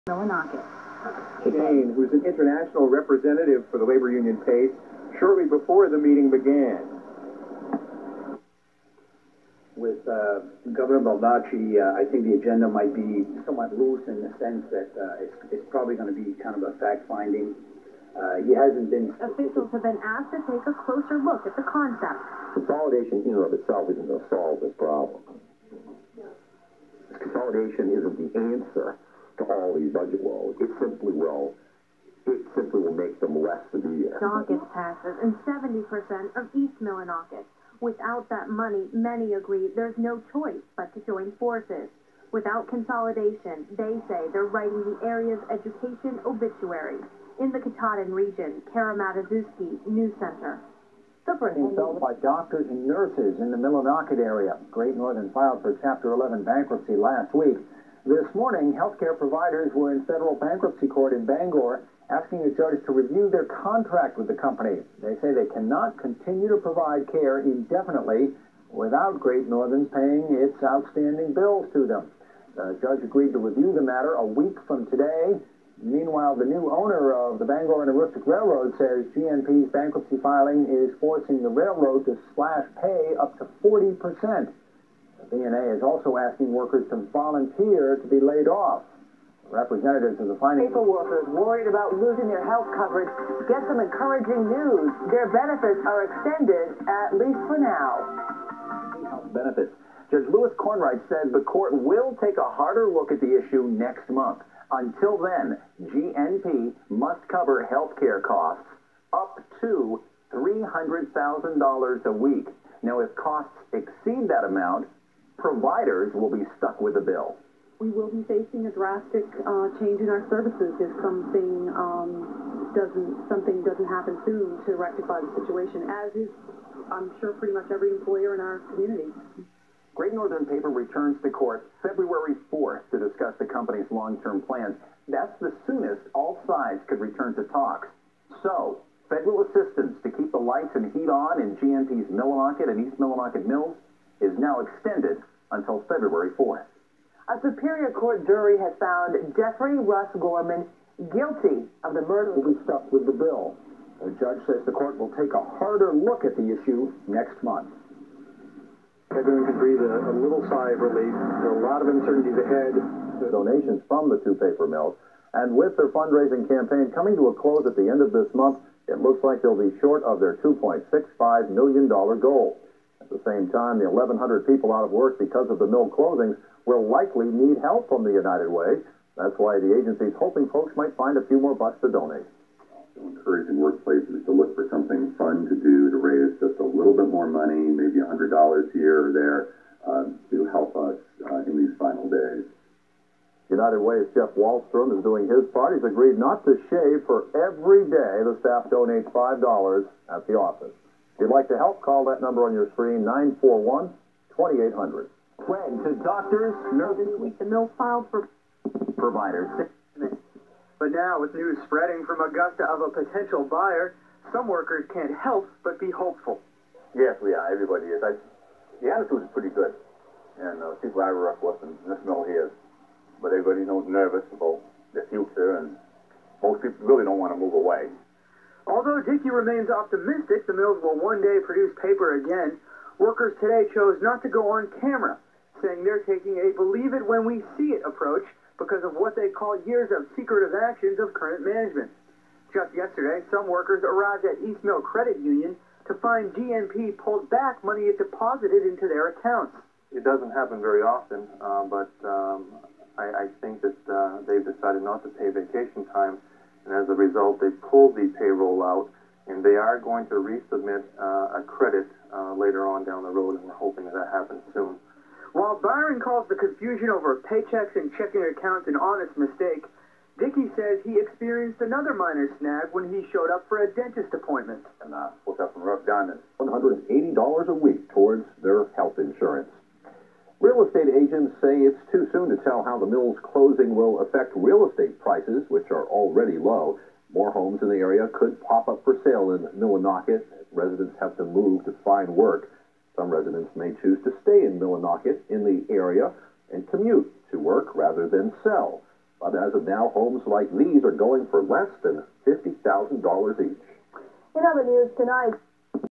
GNP Mill Closures/Bankruptcy Coverage January 14th, 15th, 16th, 2003 (WABI-TV5, WLBZ-2, WVII-ABC7)
More local TV news clips of the bankruptcies and closures of the Millinocket and East Millinocket paper mills back in January of 2003. (These were known as the "Great Northern Paper" mills. The one in Millinocket has since been demolished. But the one in East Millinocket still stands!)
Some of the reactions by area residents seen in these clips are longtime family friends.
Recorded on VHS by my dad.